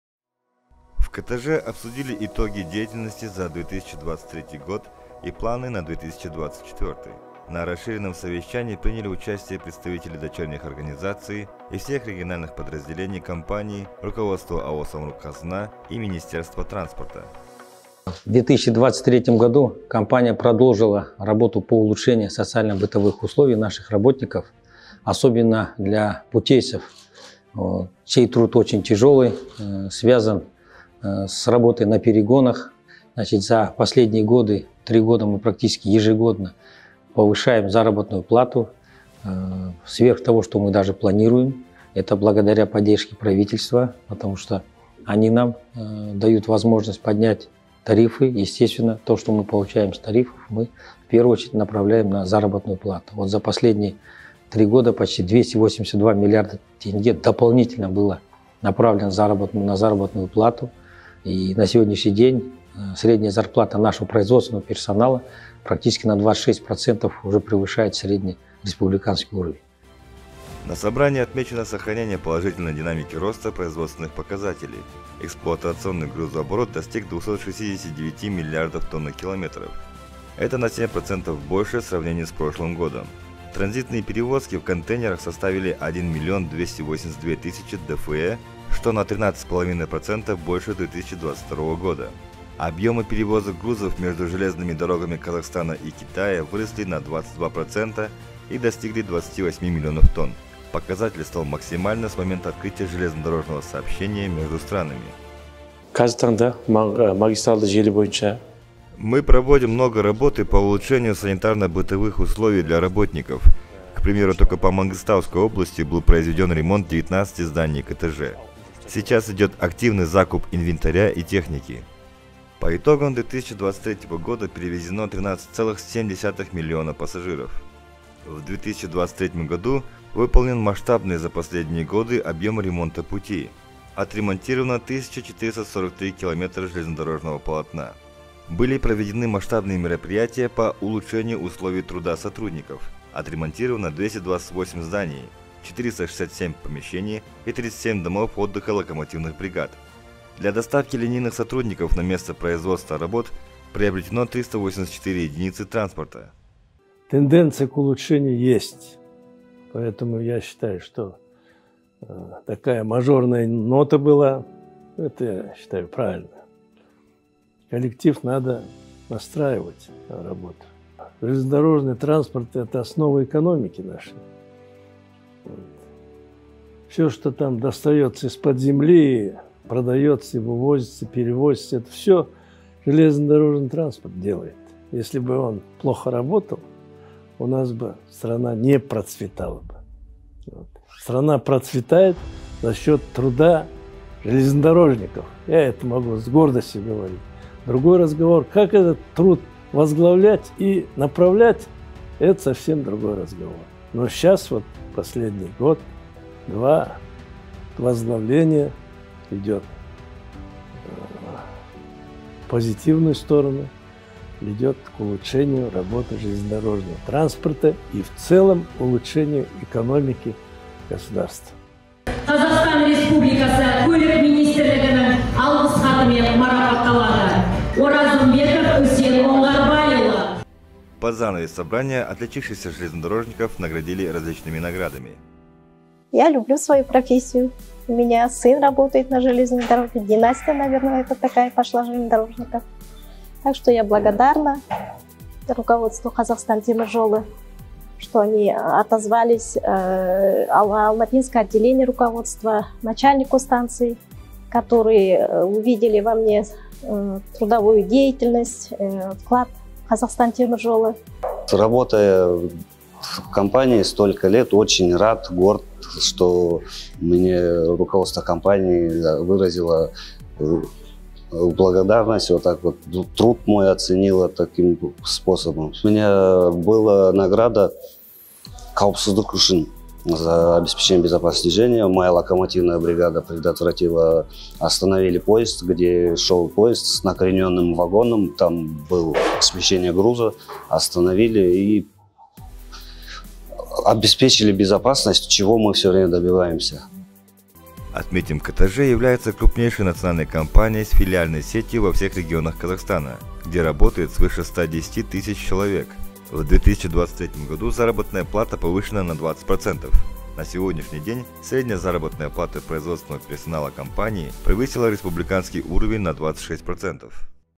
В КТЖ подвели итоги 2023 года
В АО «НК «ҚТЖ» обсудили итоги деятельности за 2023 год и планы на 2024. На расширенном совещании приняли участие представители дочерних организаций и всех региональных подразделений компании, руководства АО «Самруқ-Қазына» и Министерства транспорта.